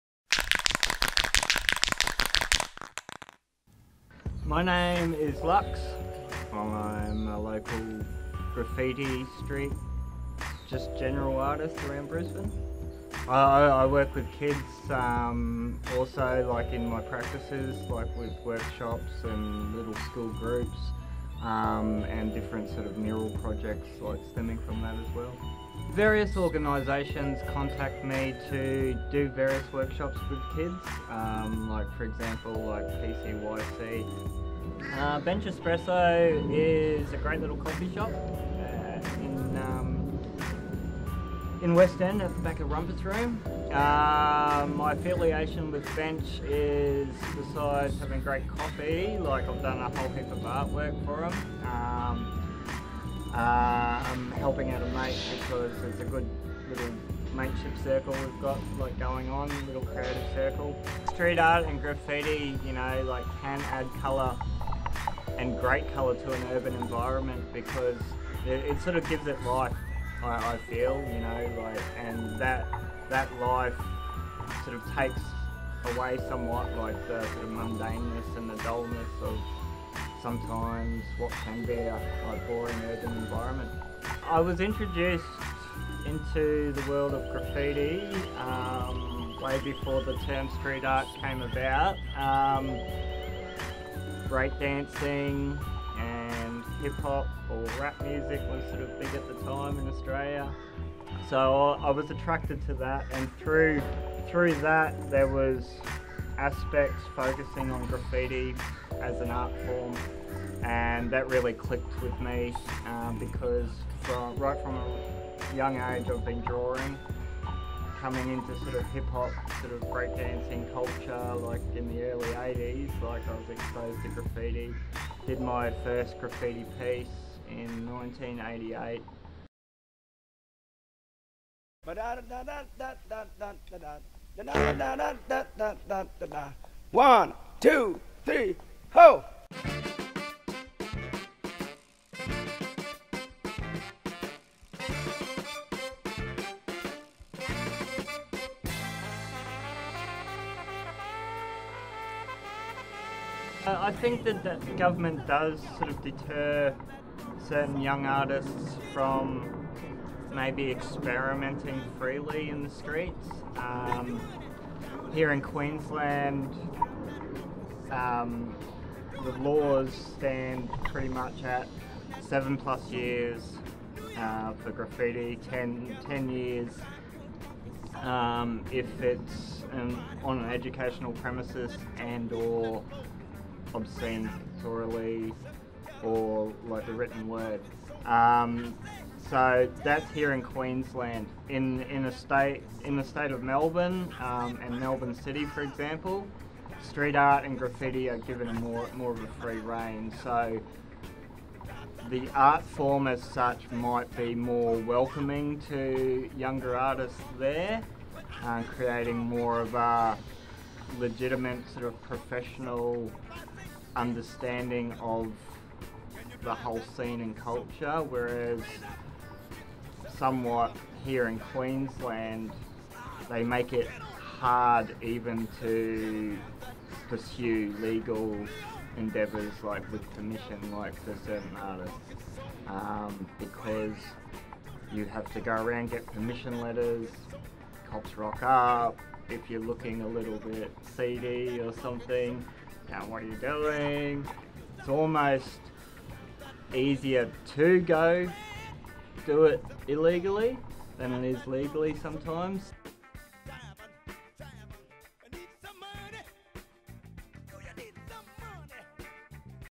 Can of Art - Documentary
A documentary about graffiti and street art in Brisbane